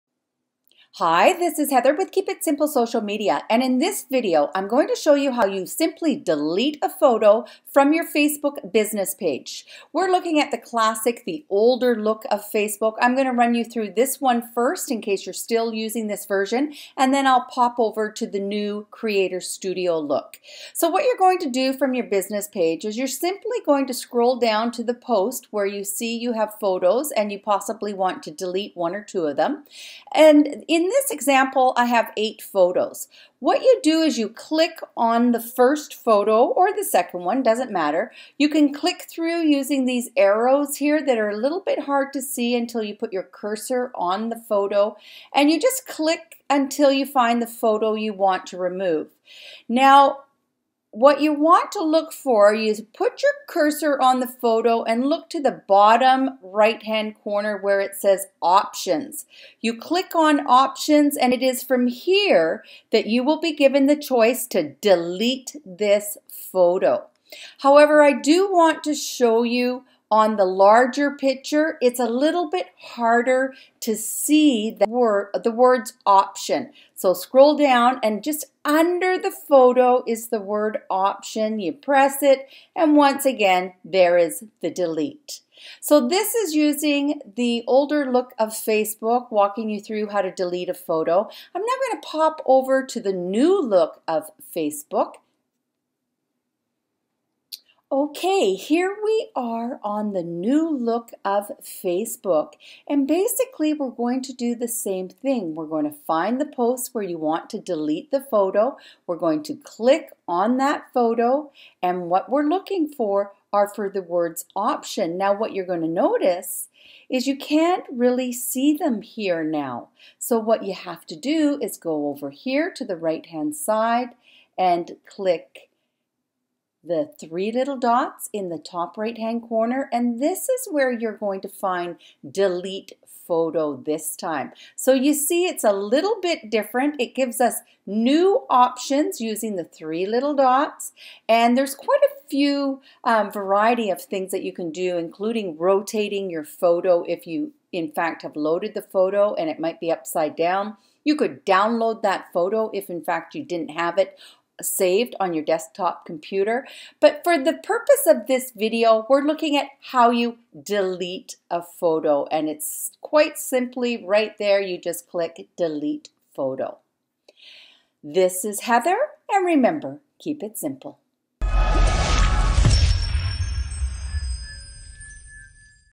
How to DELETE a Photo from Your Facebook Business Page Both New Facebook and Classic (OLD)
Hello and welcome to KISS Media TV, my name is Heather.

Often students ask me how to delete a photo from their business page.
When Facebook changed to Creator studio many of the older ways we did things changed too.
In this video, I will walk you through BOTH versions of deleting a photo.

Thank you!  Heather 💋